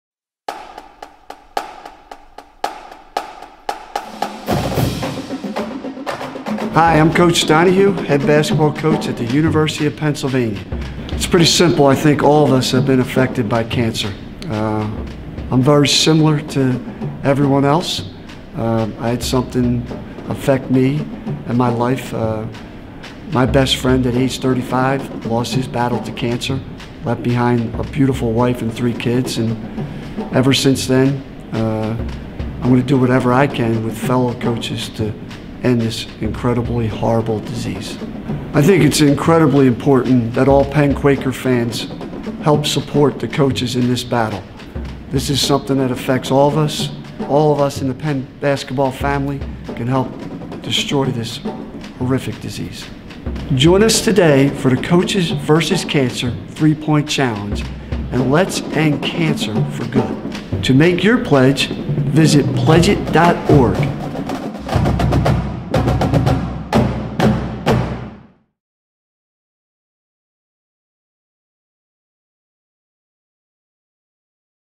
Steve Donahue: Coaches vs. Cancer 3-Point Challenge
Steve Donahue, head coach of the Penn men's basketball team, invites fans to pledge their support to the Quakers' 3-Point Challenge program to benefit Coaches vs. Cancer.

The American Cancer Society, the National Association of Basketball Coaches and the nation’s premier college basketball programs are teaming up for the 2017 Coaches vs. Cancer 3-Point Challenge! Teams from coast to coast are squaring off to see who can raise the most money from behind the arc.

All proceeds of the 3-Point Challenge will benefit the American Cancer Society and Coaches vs. Cancer.